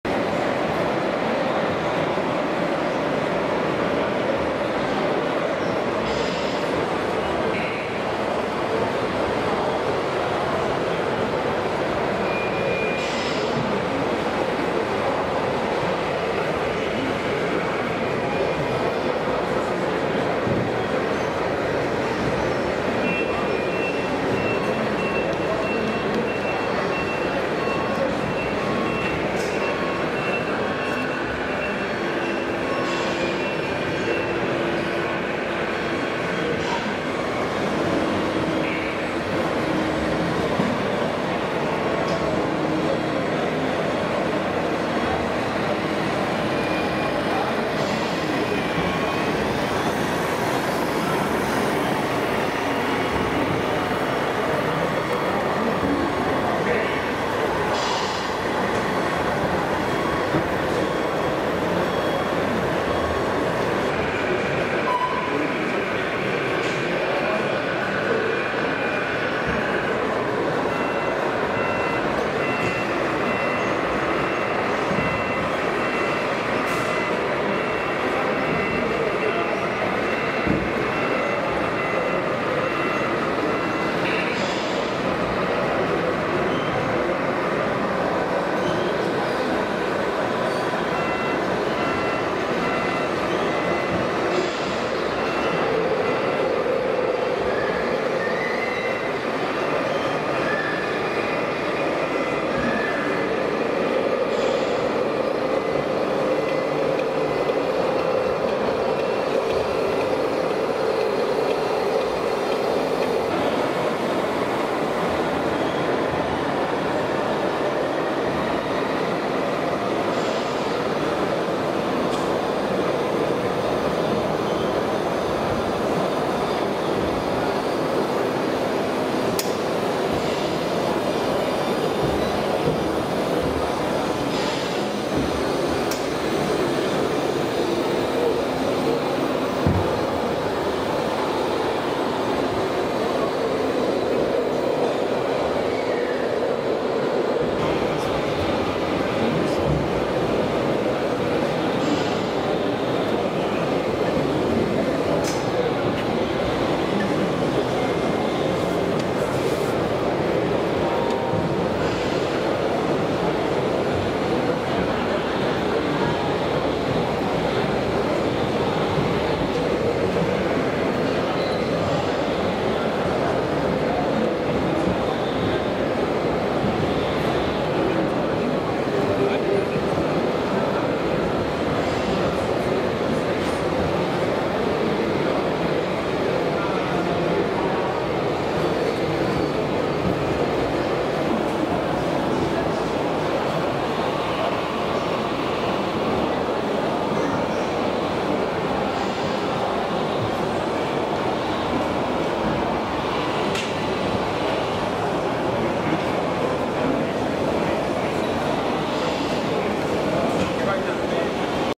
RC HEAVY TRANSPORT WITH AMAZING MODEL TRUCKS IN SCALE 1:16
RC heavy transport with scale 1/16 model trucks.
Event: Intermodellbau Dortmund April 2017